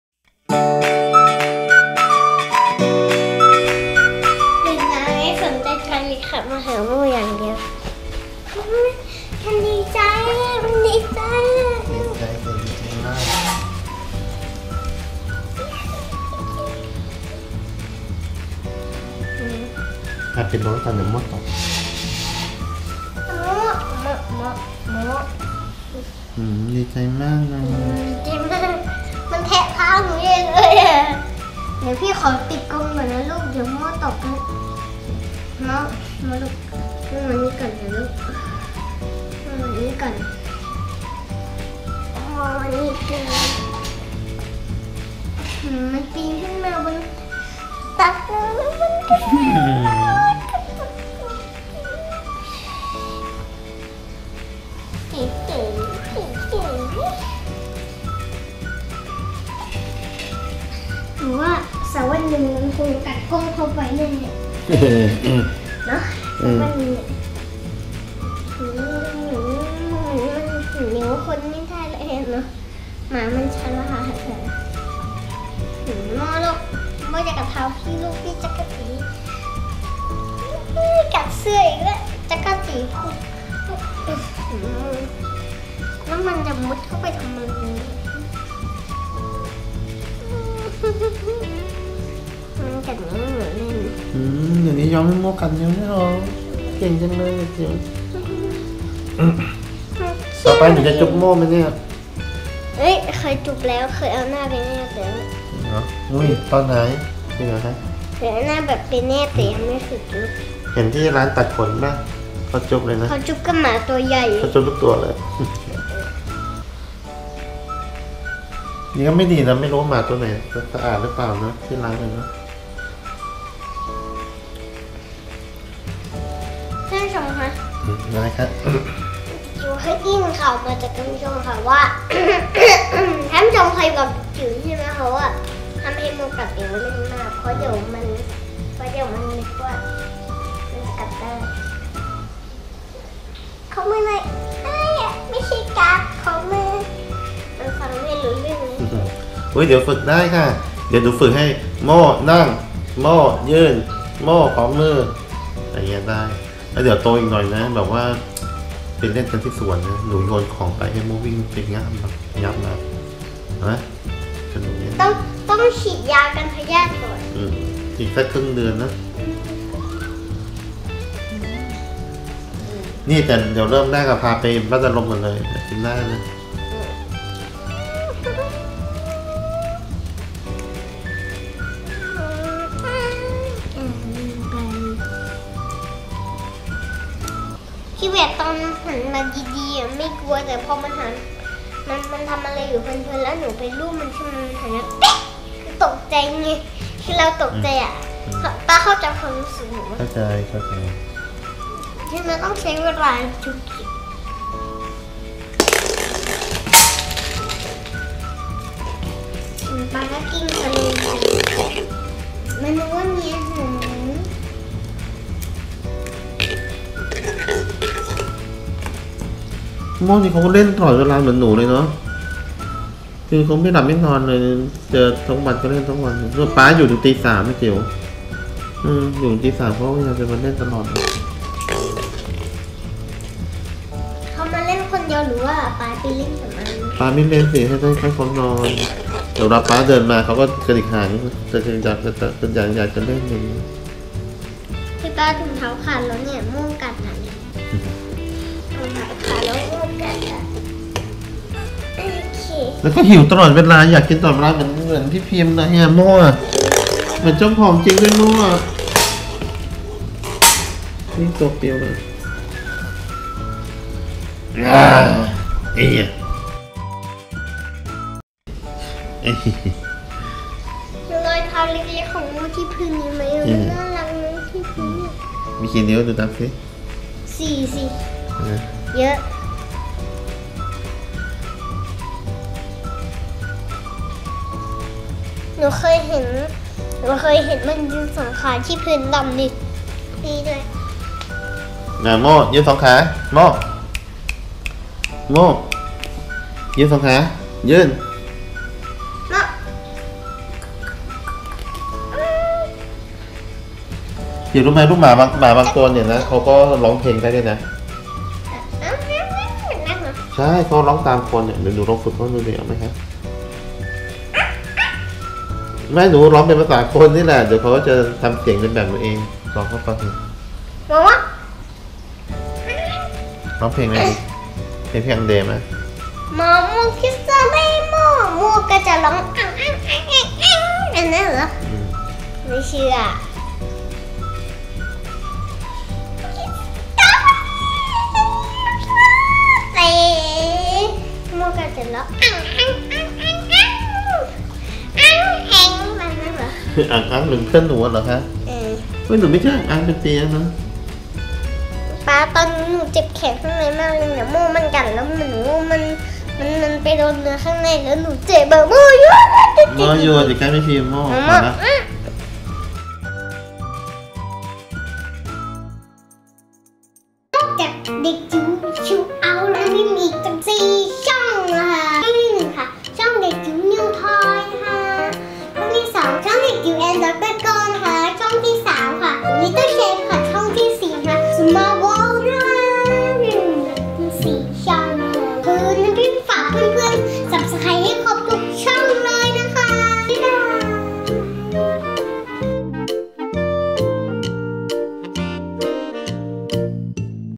เด็กจิ๋วเล่นกับโมโม่เช้าวันเสาร์ [N'Prim W325]
เด็กจิ๋วตื่นแต่เช้าทุกวันเลยช่วงนี้ เพื่อมาเล่นกับโมโม่